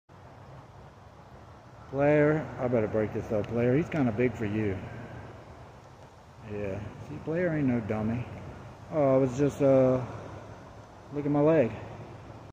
🤔 "Second Thoughts" Gray Tabby vs Cream Tabby!
The gray and white Tabby cat is Player and the big cream tabby is what I call an outskirts cat (lives on the outskirts of the group) they are feral cats that live in these woods. Player was looking like he might try something but thinking it over he had "Second thoughts". Thank goodness.